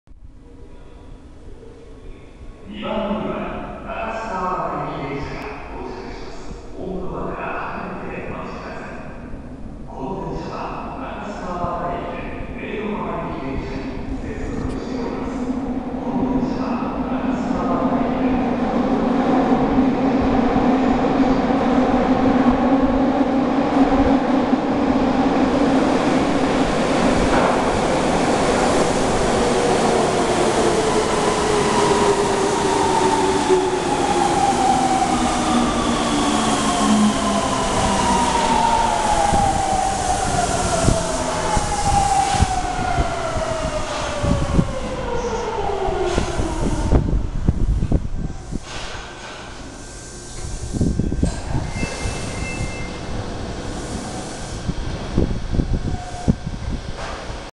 福岡市営地下鉄箱崎線(中洲川端行き)・箱崎宮前駅に到着
2019年12月17日撮影。
福岡市営地下鉄箱崎線の中洲川端行き電車(2000系)が、東区の箱崎宮前駅に到着するシーンです。